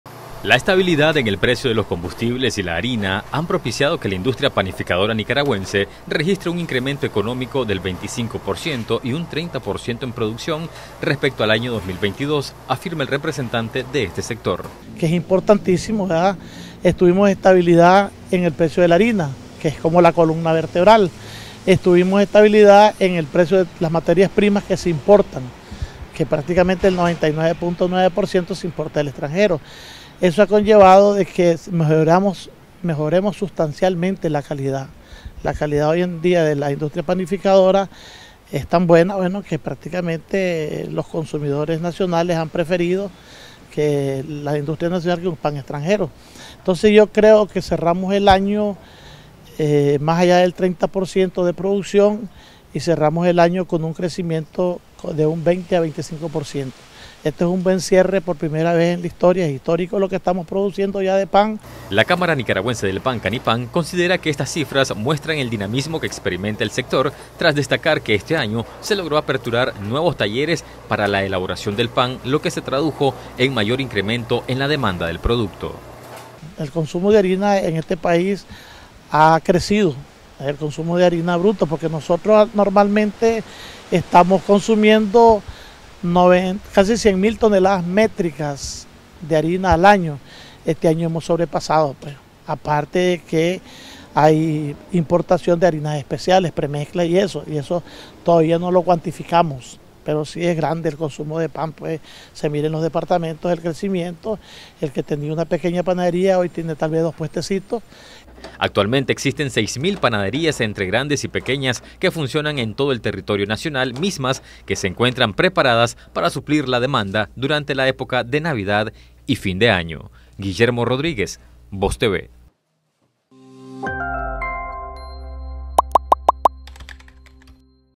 Sector panificador estima un cierre económico del 25% para este 2023
La estabilidad en el precio de los combustibles y la harina ha propiciado que la industria panificadora nicaragüense registre un crecimiento económico del 25 por ciento y un 30 por ciento en producción, respecto al año 2022, afirma Juan Pablo Estrada representante de este sector.